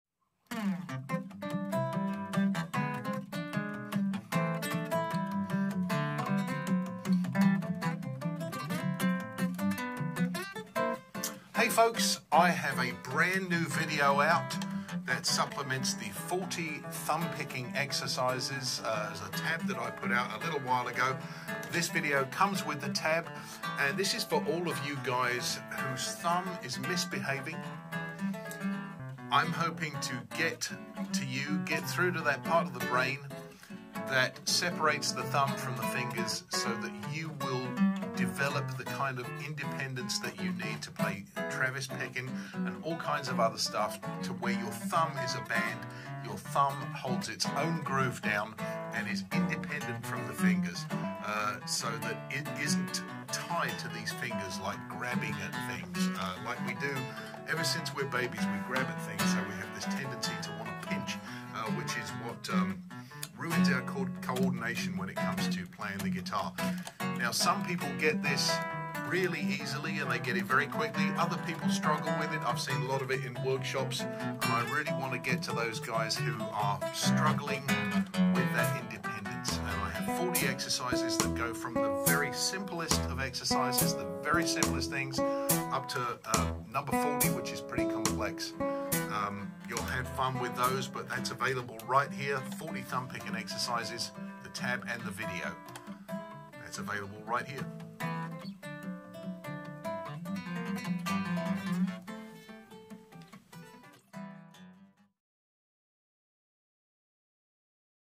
"40 Thumb Independence Exercises" with Richard Smith
Over one hour of exercises. 8 short videos of 5 exercises that will develop the independence you need for your thumb to have its own groove.